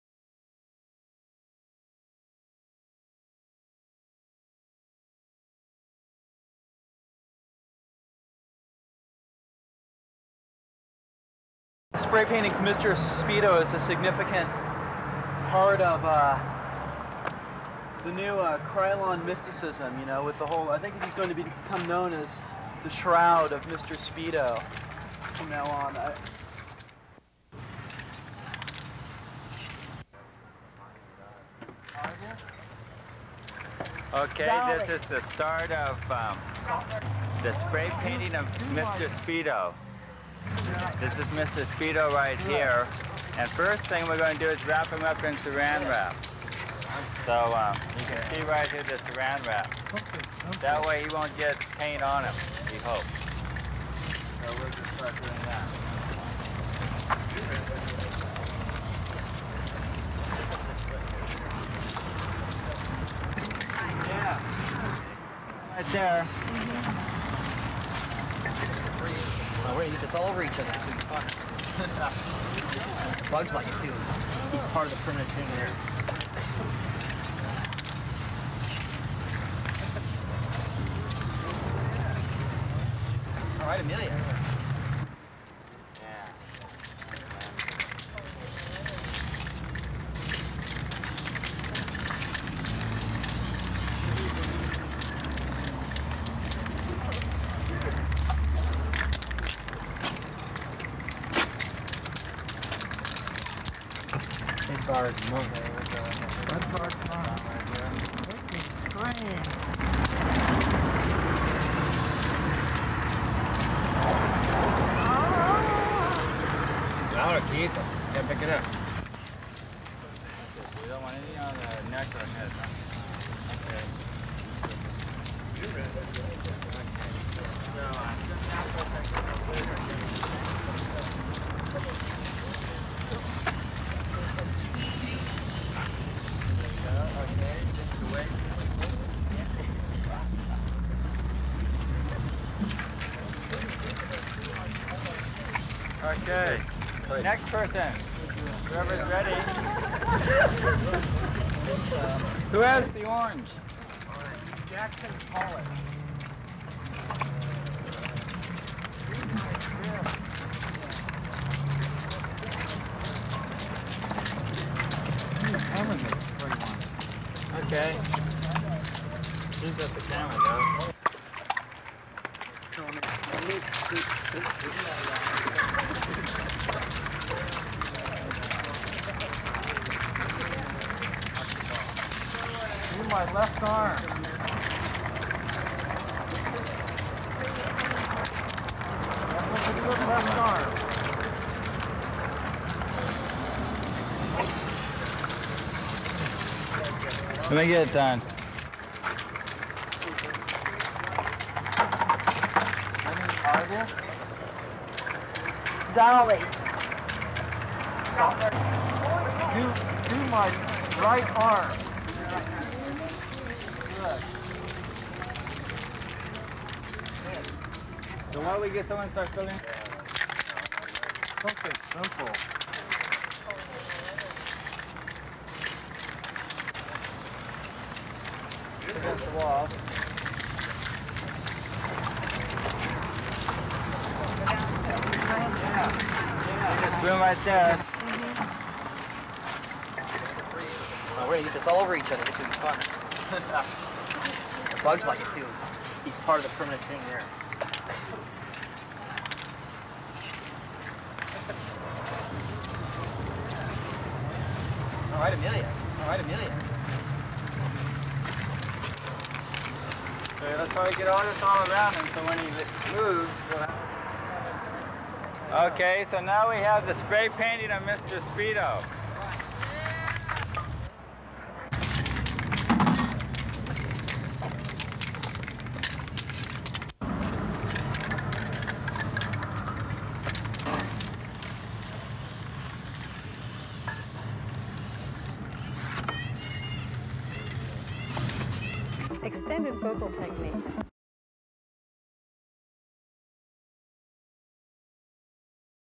The Spray Painting of Mr. Speedo
presents this 1980's performance art piece that took place at Leonard's Artspace in Northern California, featuring Keith Longcor as Mr. Speedo